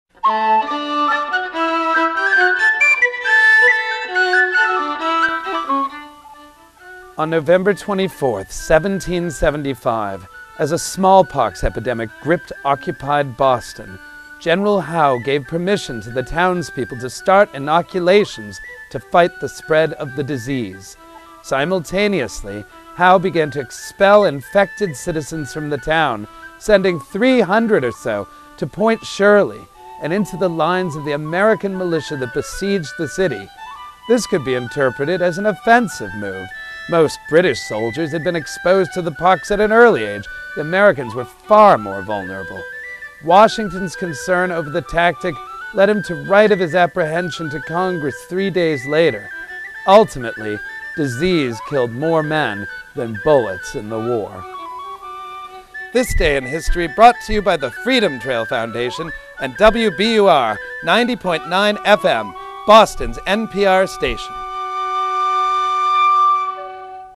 This Day In History - November 24 - Freedom Trail Foundation
Freedom Trail Player Jeremiah Poope recounts a smallpox outbreak and possible biological warfare for This Day In History.